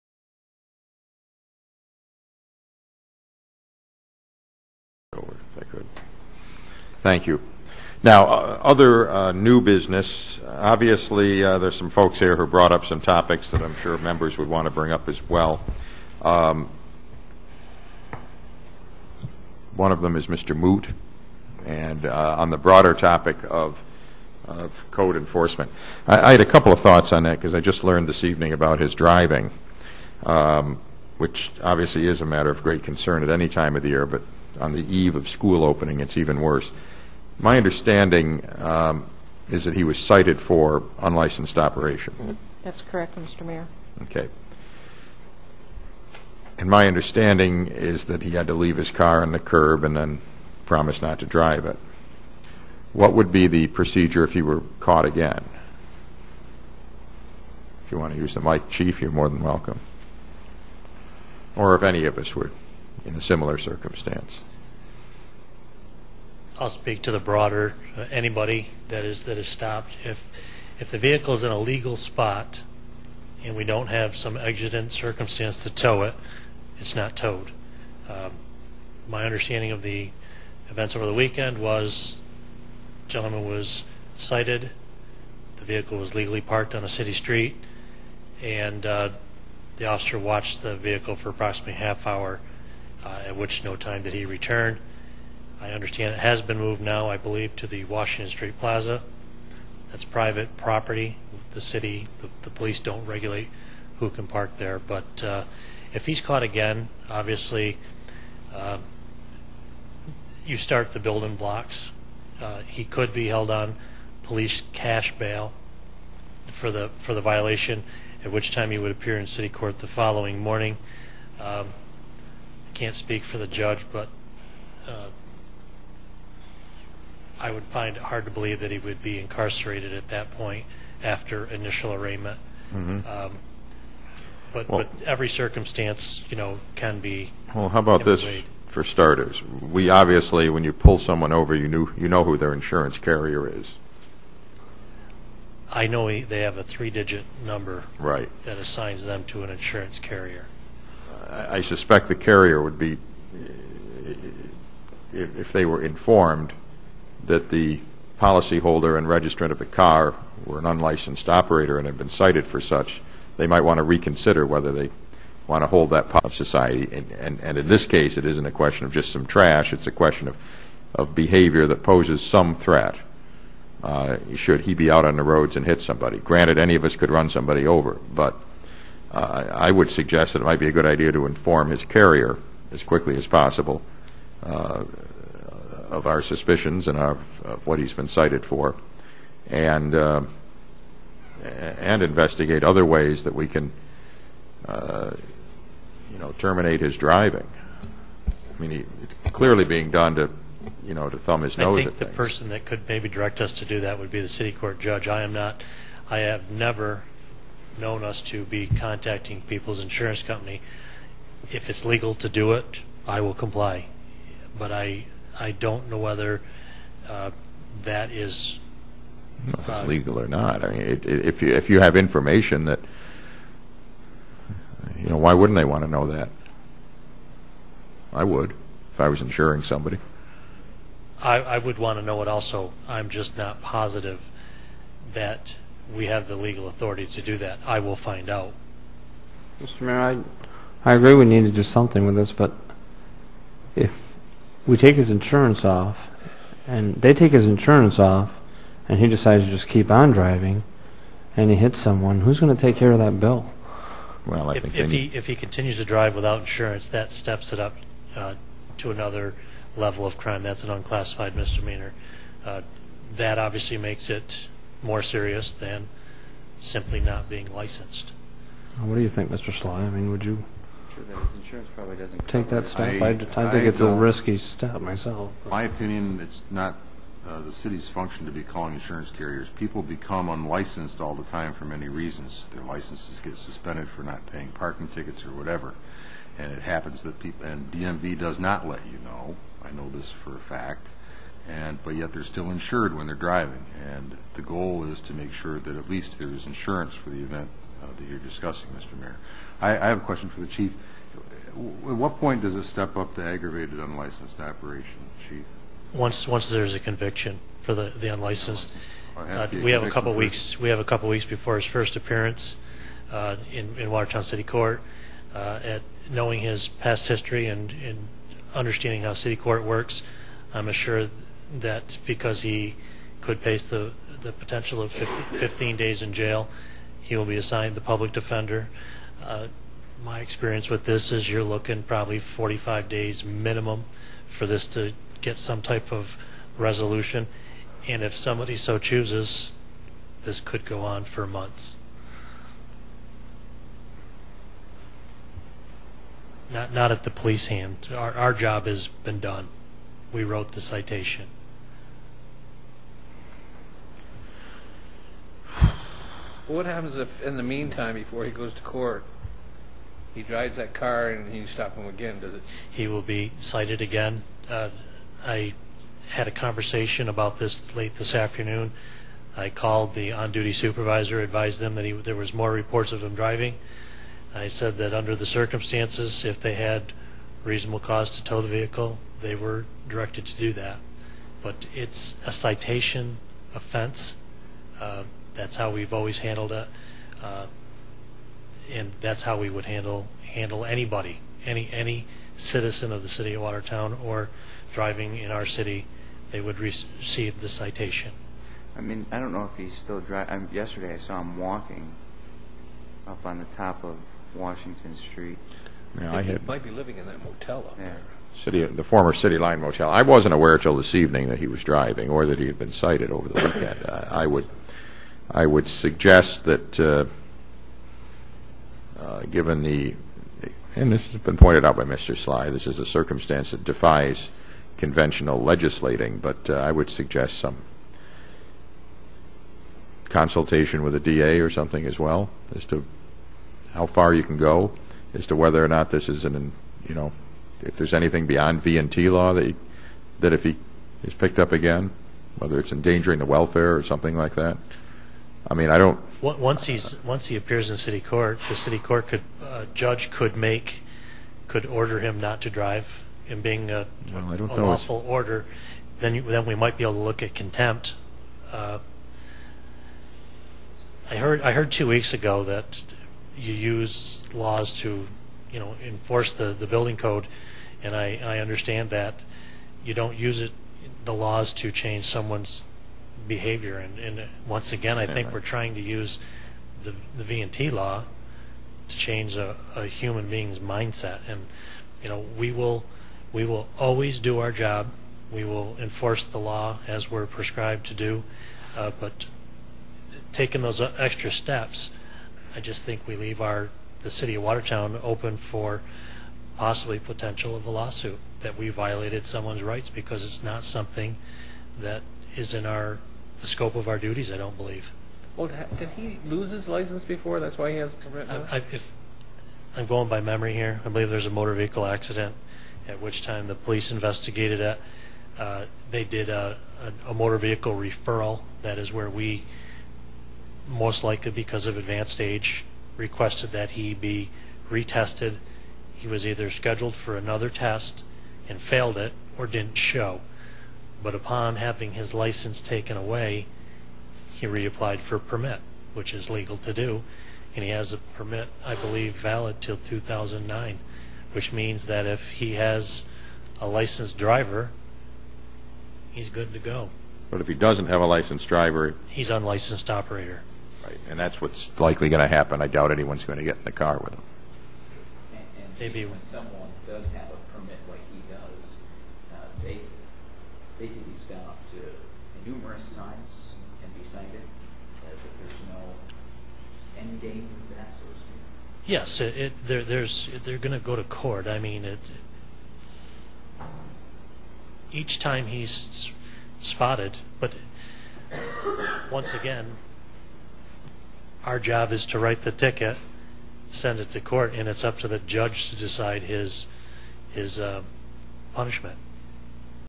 Watertown City Council Sept. 4, 2007
Discussion about city resident who lives on Ward Street.  Watertown N.Y. City Council.  Sept. 4, 2007